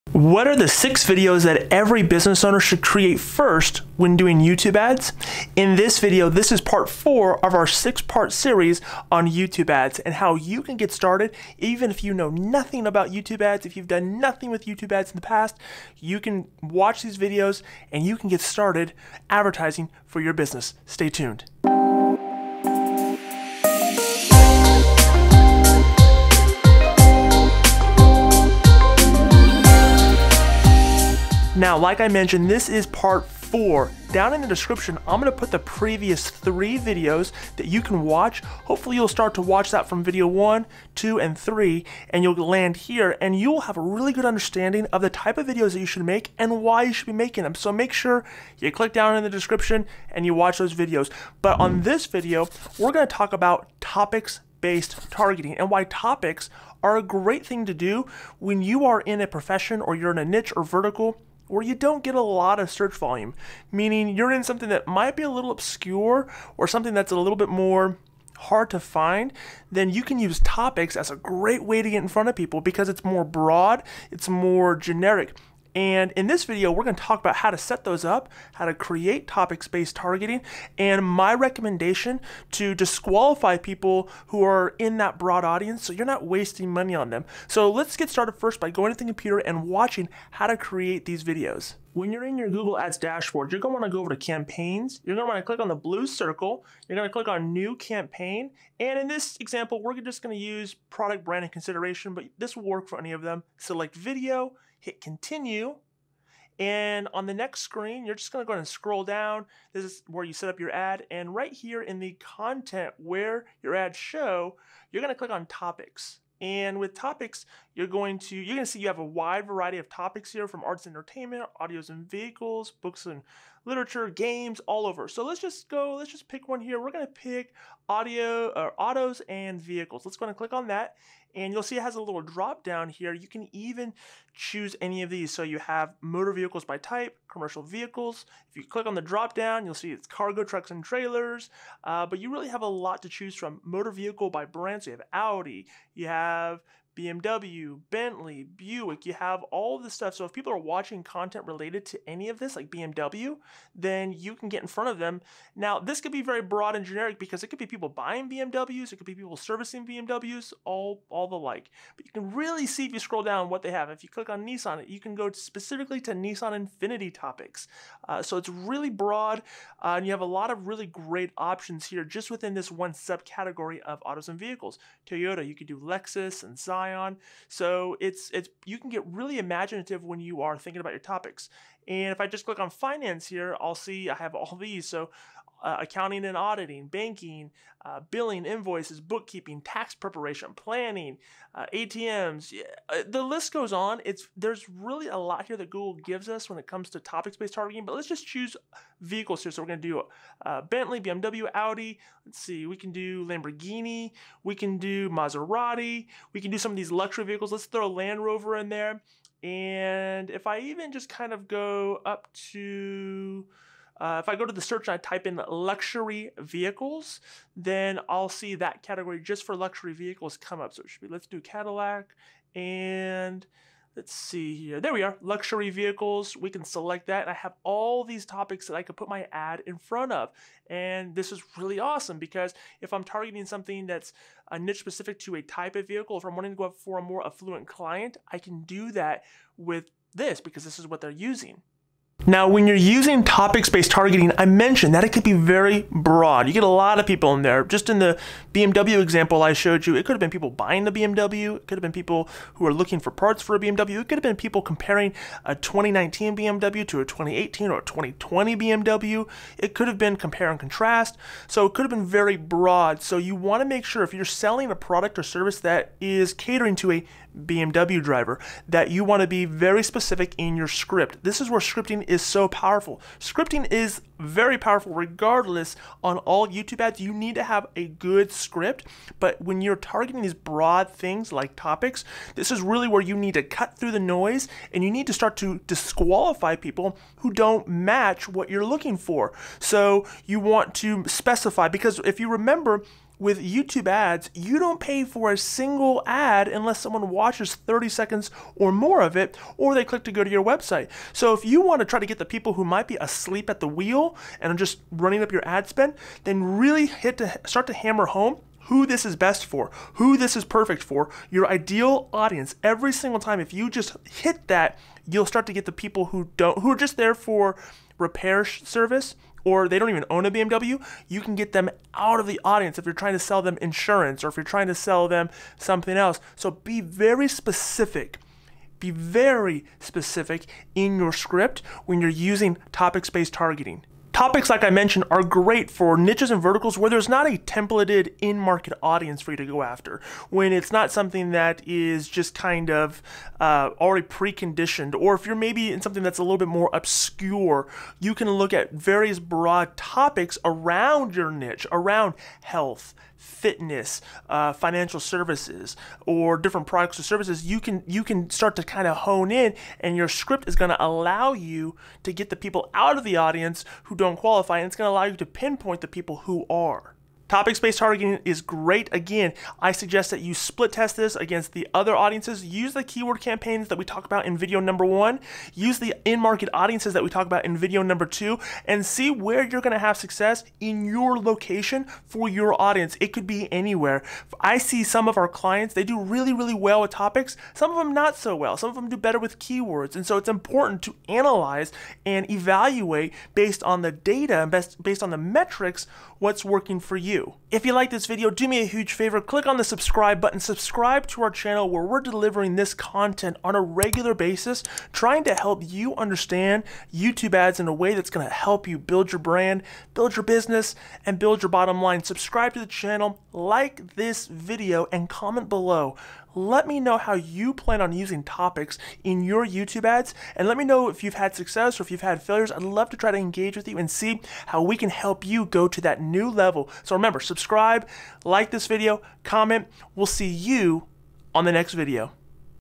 How To Use Topics Targeting With YouTube Ads
Learn how to use topics targeting in your YouTube ad campaigns.

This training is part 4 of our 6-part series on "The First Ads You Should Start With On YouTube"